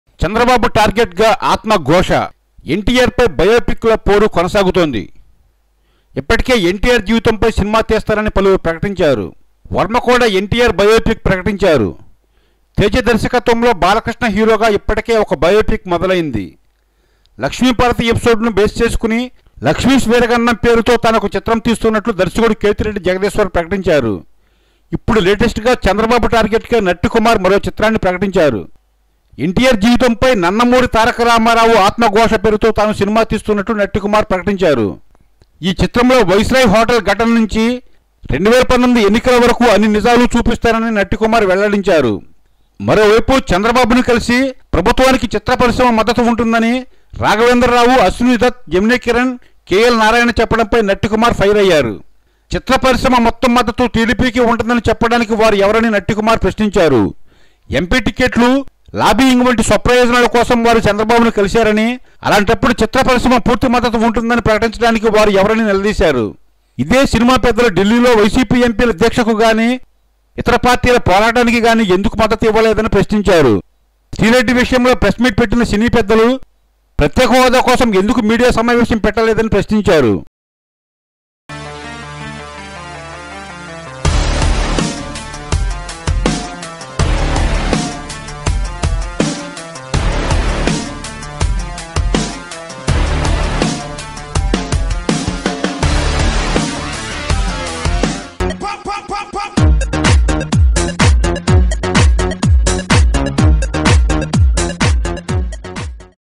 చంద్రబాబు టార్గెట్ గా త్వరలో ఎన్.టి.ఆర్ ఆత్మఘోష| Another NTR Biopic Movie With Chandrababu Target..!
Tollywood Producer Natti Kumar preparing to Produce Another Bio Pic Movie on NT Rama Rao. Natti Kumar Said that Movie Starts on the base Chandrababu Naidu backstabbing Issue to Upcoming Elections.
Watch this video to know full details abour rhe movie
Amaravathi Media